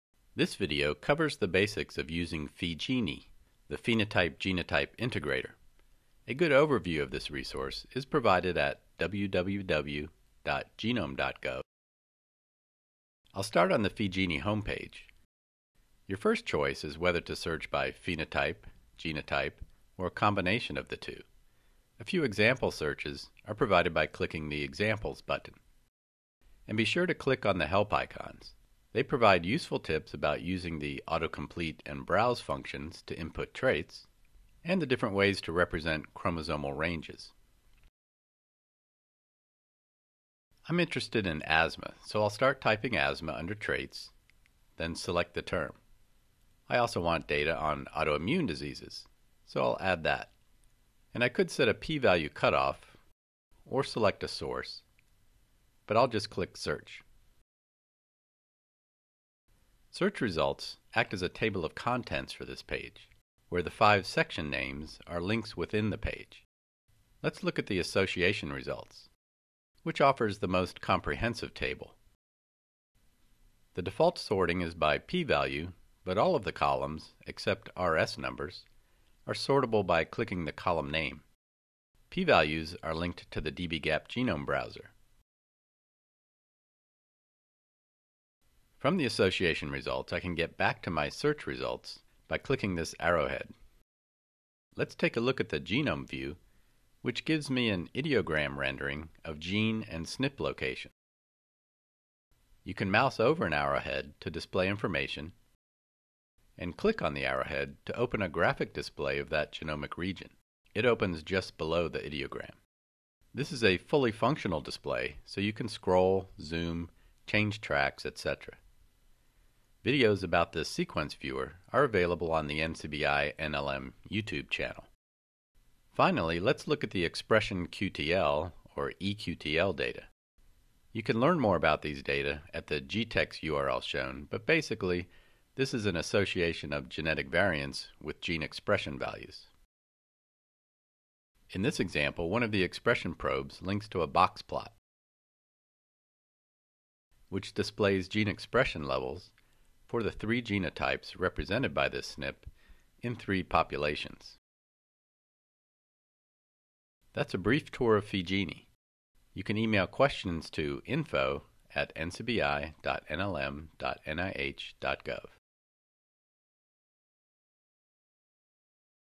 PheGenI: The Phenotype-Genotype Integrator
A January 2013 update to the PheGenI resource of dbGaP. Changes include: autocomplete search for Traits; example searches; source choice; and enhanced Genome View.